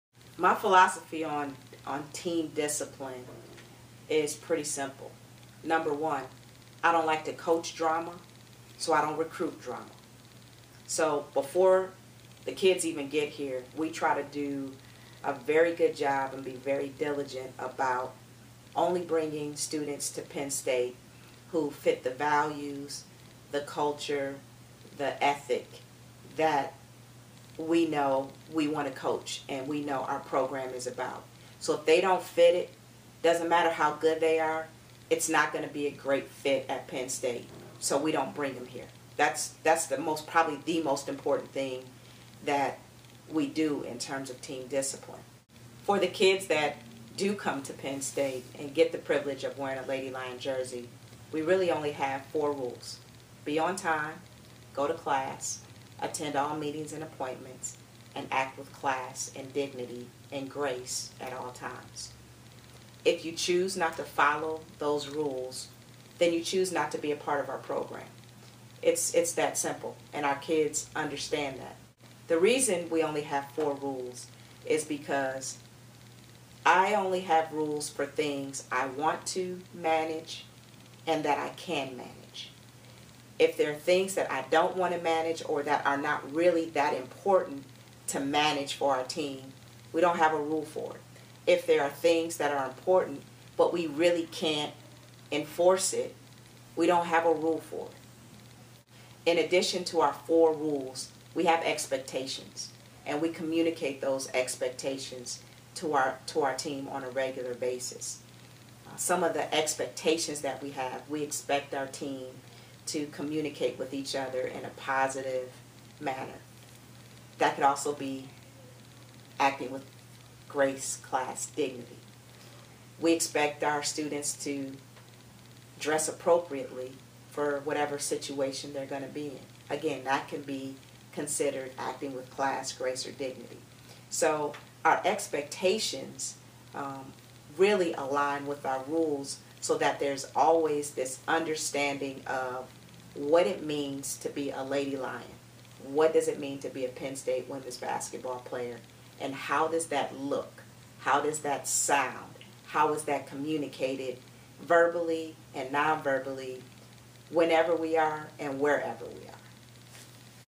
Coquese Washington: Team Discipline
Coquese talks about her approach to team discipline.